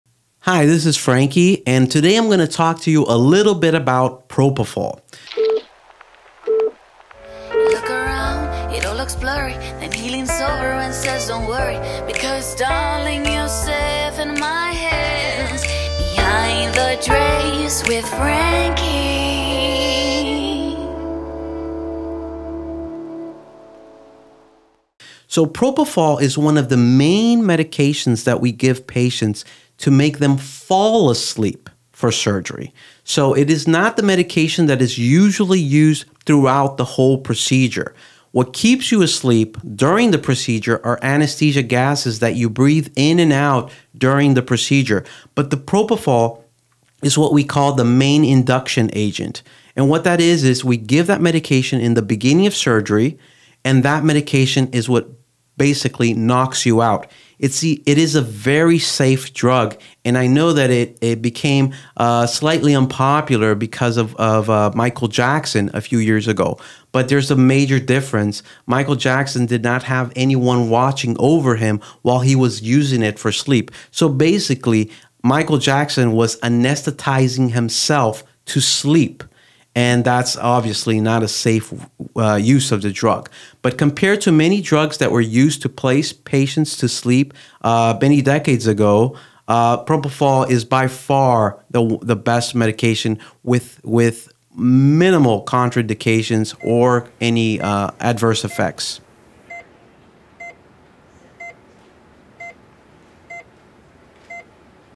Going to Sleep with Anesthesia
Hey there, YouTube fam! 🎉 Have you ever wondered how you go to sleep with Anesthesia?  Join us on this captivating journey as we discuss the use of propofol in anesthesia! 💉💭

In this video, we'll discuss how we use propofol, the remarkable medication used to put you in that sweet, dreamy state during surgery.

00:00 Intro
00:24 Propofol